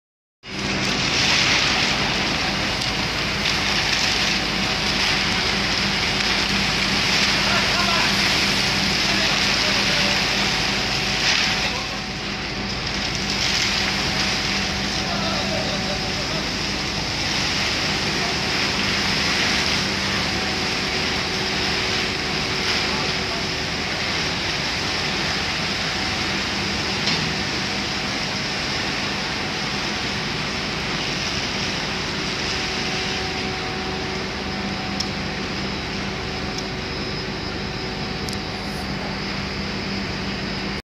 Sacado de Plastico de loc. CRRC Qishuyan CDDA5A1 Nº9418 en el Puerto
En horas de la mañana tras haber sido descargado del barco BBC VESUVIUS y encarrilada en Terminal 5 E en nuestro puerto de la Administracion General de Puerto (AGP) para luego ser trasladada a Terminal 5 para ser dejada a ordenes.

Las mismas tienen como destino Alianza (Linea San Martin) para Trenes Argentinos Cargas de ese sector para sus cargueros en nuestro pais